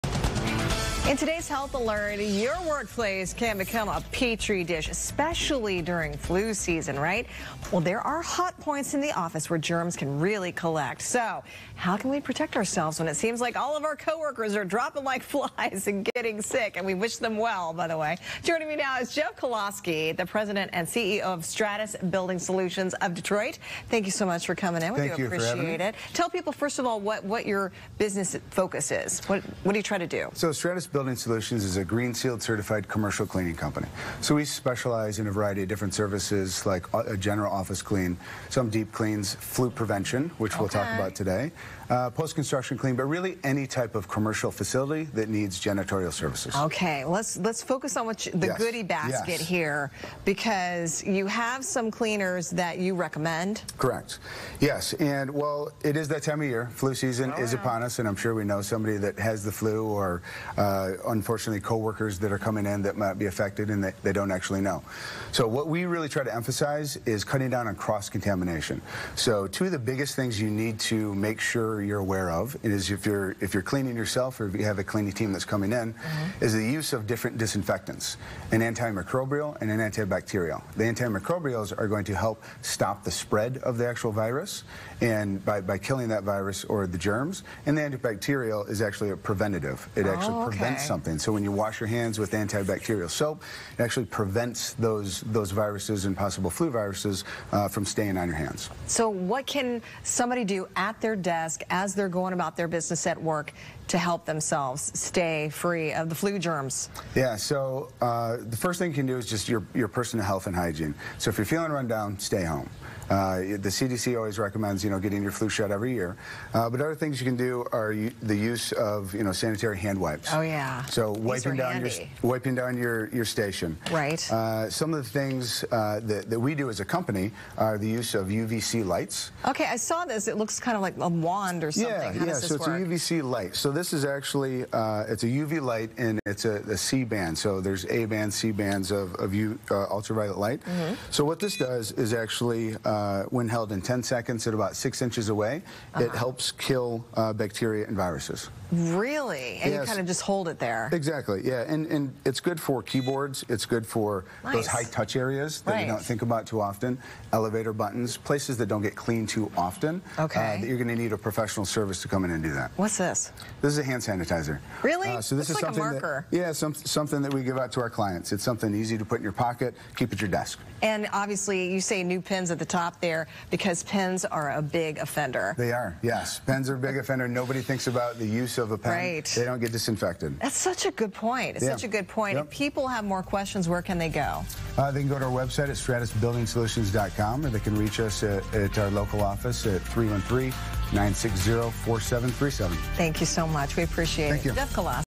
The Germiest Spots in the Office WMYD 20 Detroit
Stratus Building Solutions of Detriot speaks to WMYD 20 News about what the gerimest spots in the office are and what cleaning techniques will keep you healthy this flu season!